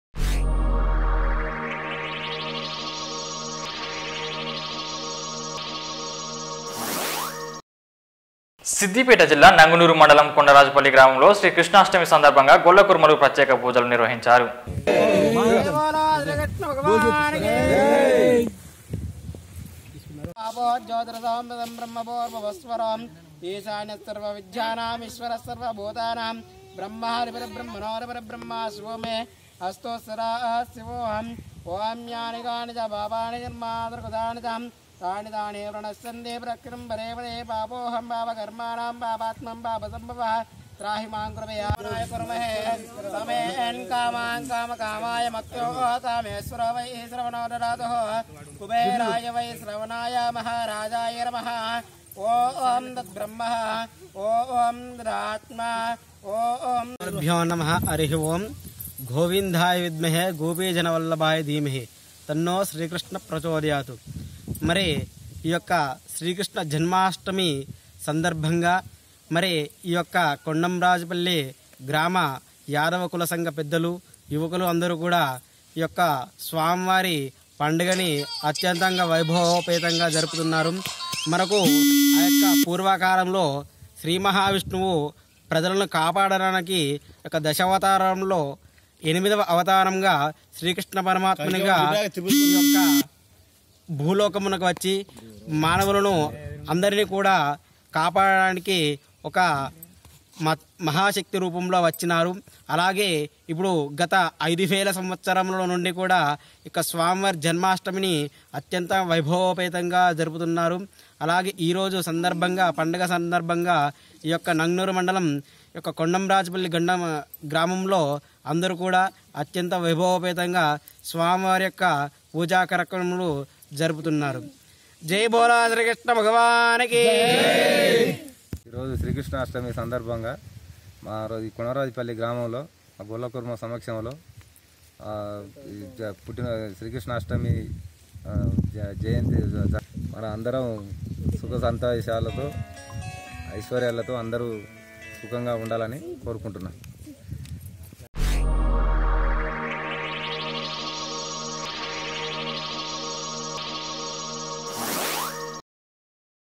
శ్రీకృష్ణ అష్టమి సందర్భంగా గొల్ల కుర్మలు ప్రత్యేక పూజలు |TS24 News
సిద్దిపేట జిల్లా నంగునూరు మండలం కొండంరాజుపల్లి గ్రామంలో శ్రీకృష్ణ అష్టమి సందర్భంగా గొల్ల కుర్మలు ప్రత్యేక పూజలు  నిర్వహించారు